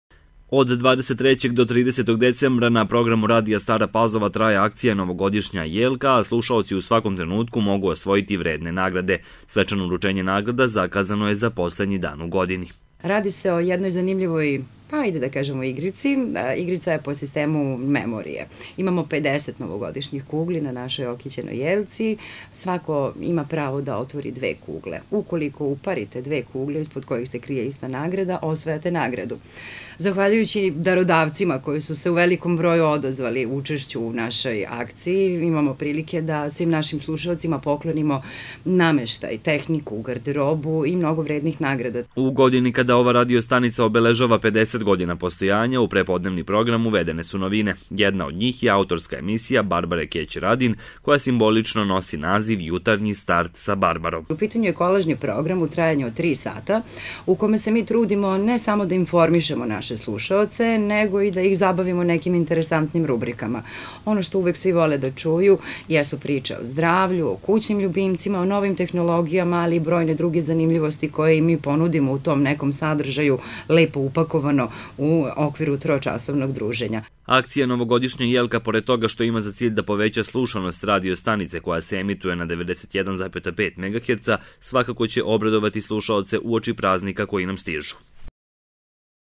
U toku akcija Novogodišnja jelka
Radio Stara Pazova spremno dočekuje novogodišnje i božićne praznike, pa je tako autorka emisije „Jutarnji start sa Barbarom“ odlučila da okiti novogodišnju jelku svetlečim kuglama i vrednim nagradama, a slušaci će ovom priliku imati mogučnost da rskite jelku i tako osvoje vredne nagrade.